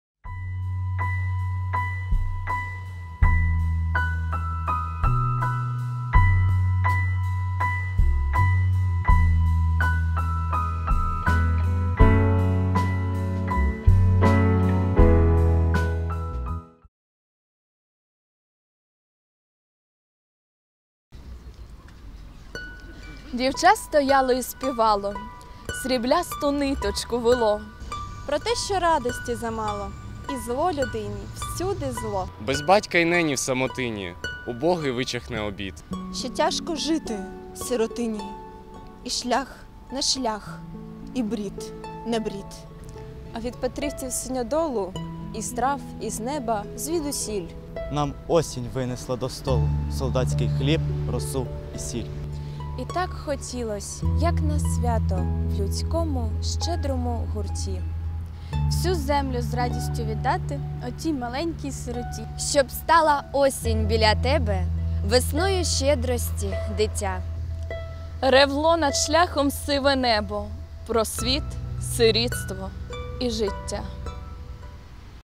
PM TV: Андрій Малишко "Дівча". Читають студенти ЧНУ ім. Петра Могили
14 листопада Україна відзначає 104 роки від дня народження українського поета Андрія Малишка.
З цієї нагоди студенти-журналісти прочитали один із віршів поета.